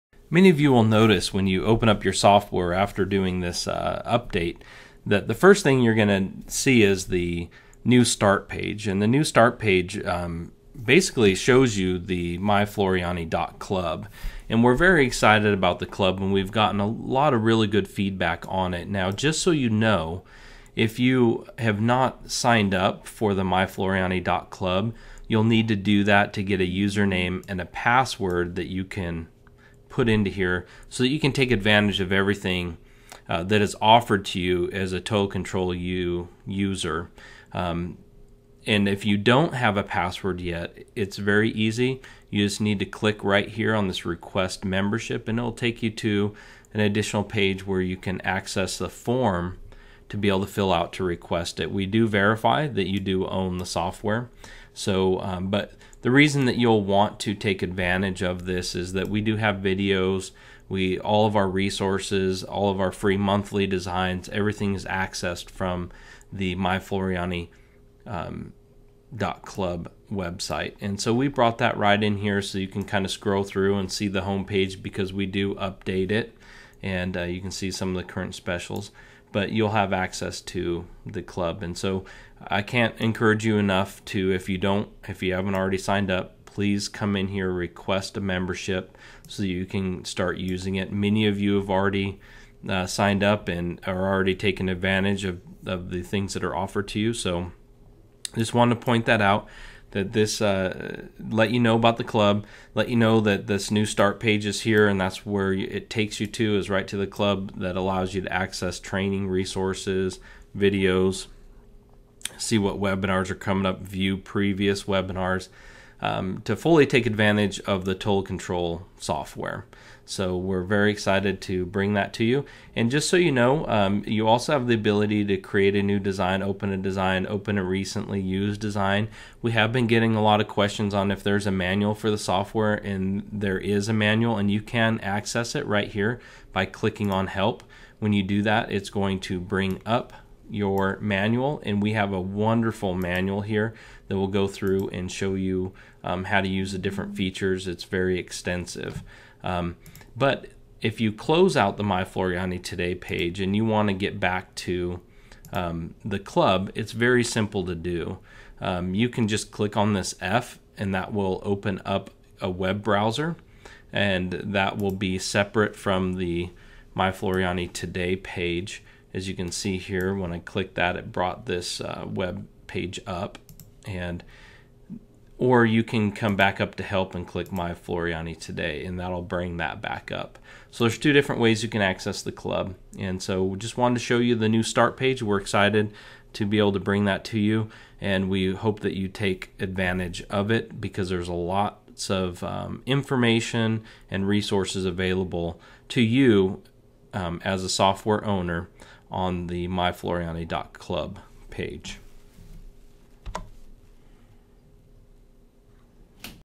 FTC-U Start Page - 2752
Floriani Total Control U has a new start page that appears when you open the software.  From this new start page you will be able to access the MyFloriani.Club site and create a new design page, open a design, open a recently used design, see what's new with Floriani products, and access the help manual.